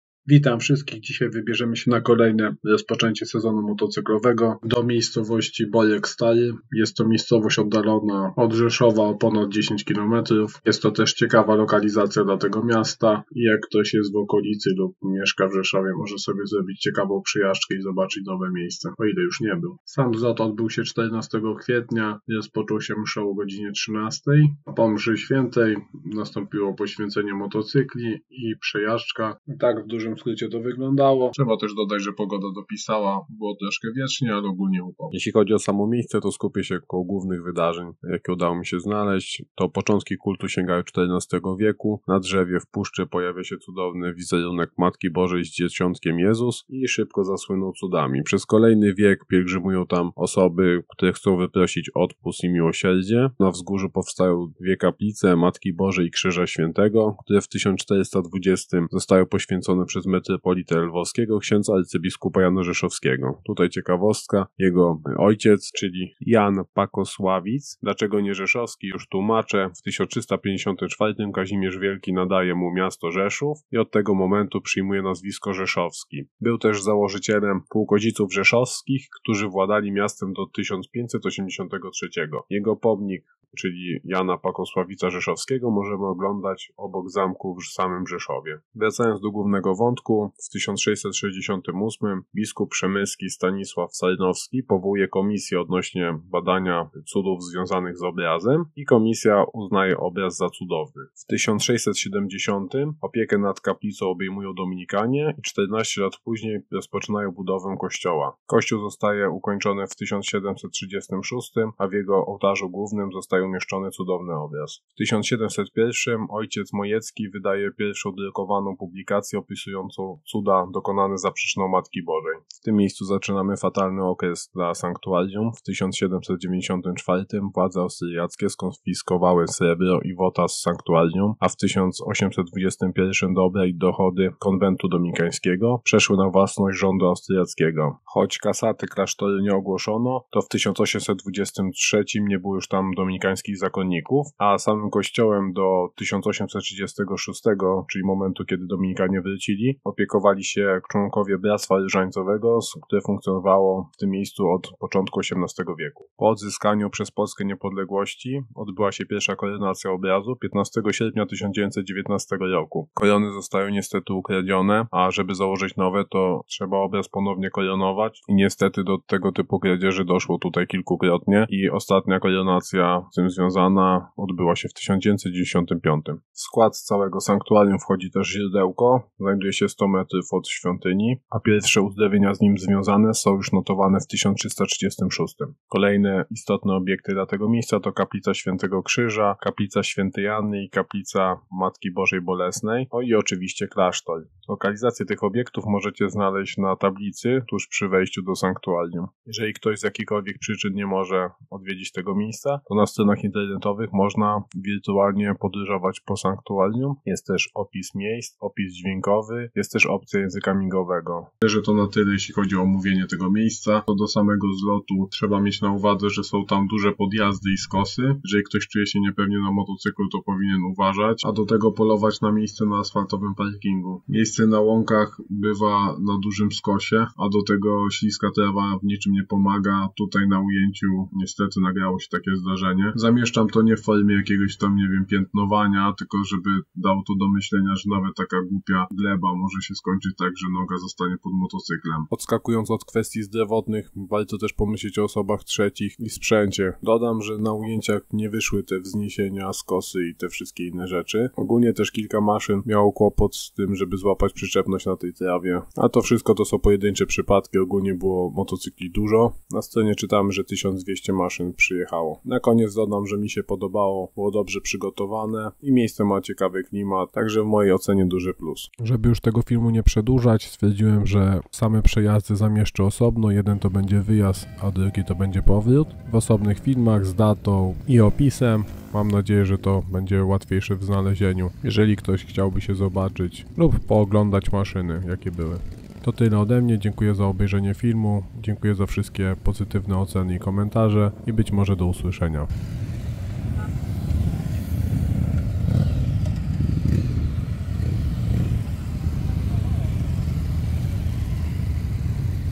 Rozpoczęcie Sezonu Motocyklowego w Borku Starym – 14.04.2024
Witamy!
Zapraszamy do obejrzenia filmu z Rozpoczęcia Sezonu Motocyklowego w Borku Starym k. Rzeszowa, które odbyło się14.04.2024. Impreza zorganizowana została przez Forum Podkarpackich Motocyklistów oraz Dominikanów z Borku Starego, rozpoczęła się o godz. 13.00 mszą św. przy ołtarzu polowym dla motocyklistów rozpoczynających sezon. Po mszy św. ponad 1000 motocyklistów wzięło udział w Procesji motocyklowej ku czci Miłosierdzia Bożego z prośbą o bezpieczną i mądrą jazdę oraz miłosierdzie dla tych, którzy na drogach ucierpieli lub zginęli. Na tym rozpoczęciu sezonu pojawiło się ponad 1200 maszyn. Więcej informacji dotyczących miejsca, organizatorów i wydarzenia znajdziecie w linkach poniżej jak i samym filmie.